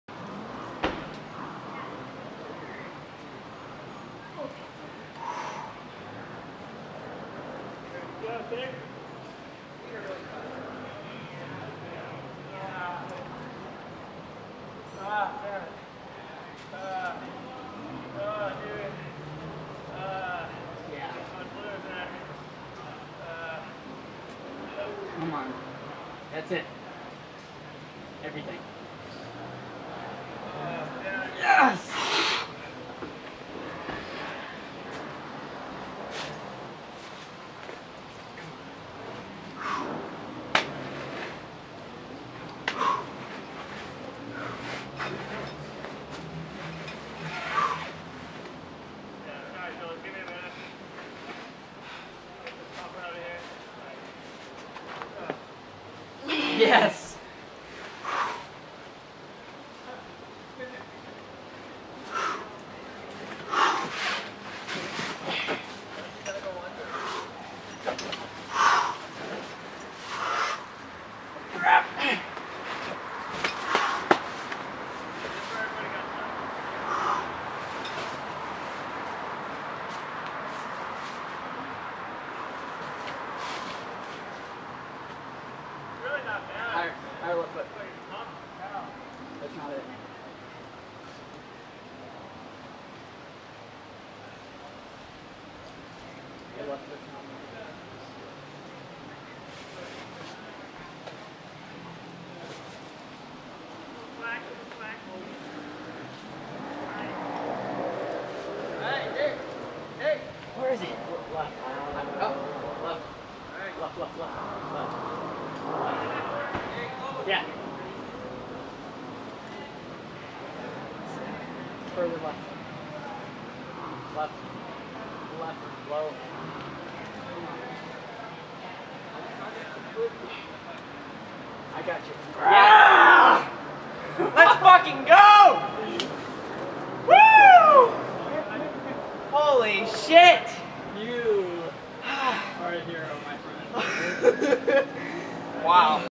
Rumney Bouldering: "Laws of Gravity" V9
The line in between "Escape From Reality" and "Choss is Reality". This is a fun and powerful roof climb that utilizes a few fingerlocks and a hard footcut that leads to a v5 outro sequence and a topout that is usually wet, hence the epic.